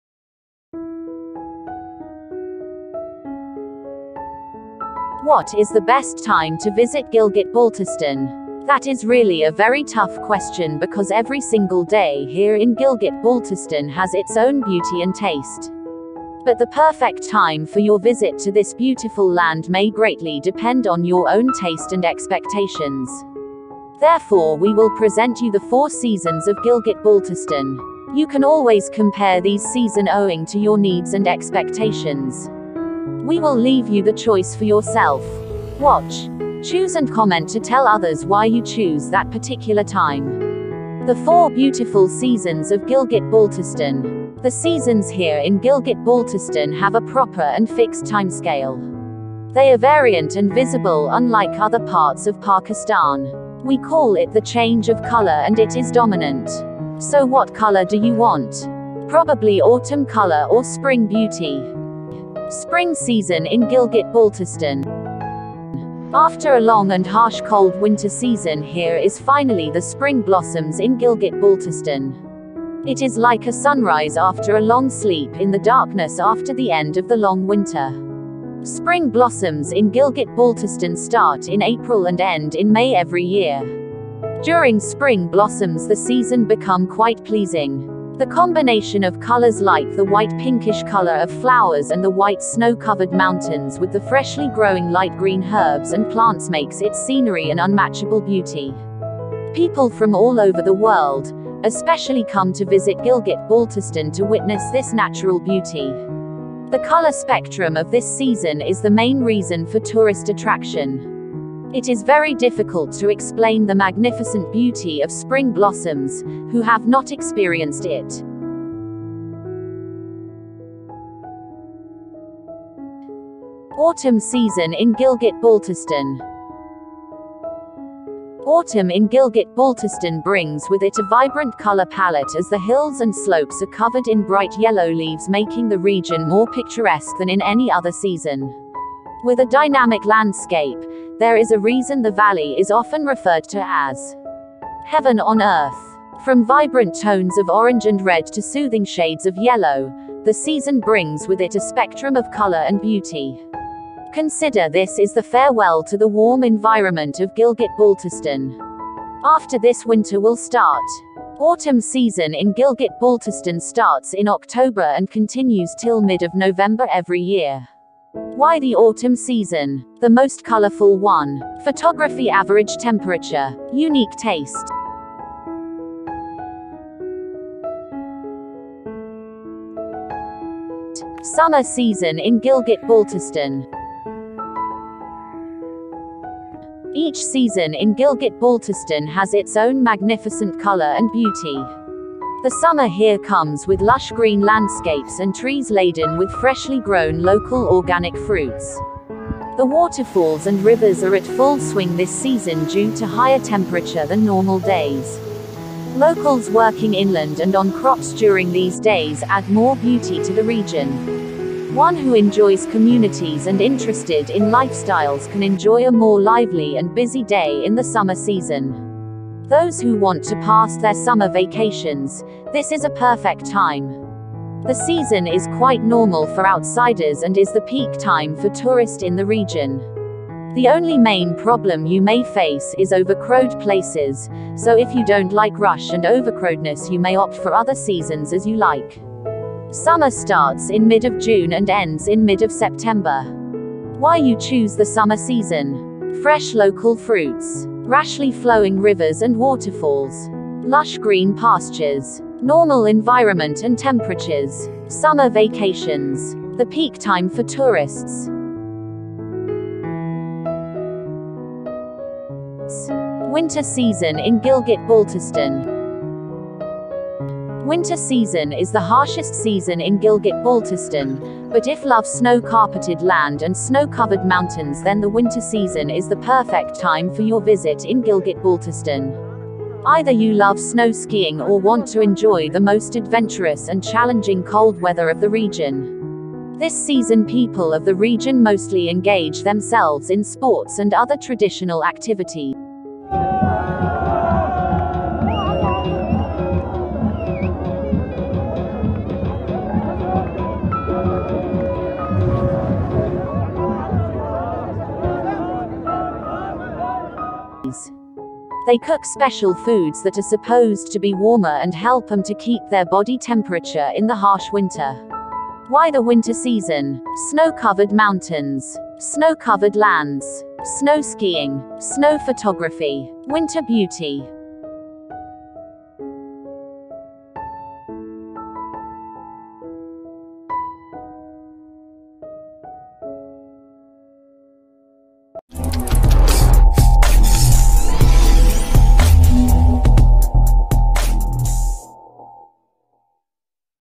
The Four Beautiful Seasons of Gilgit Baltistan.| Spring, Autumn , Summer , Winter
What is the best time to visit Gilgit Baltistan?
The Four Beautiful Seasons of Gilgit Baltistan.
Spring , Autumn ,Summer and Winter season in Gilgit Baltistan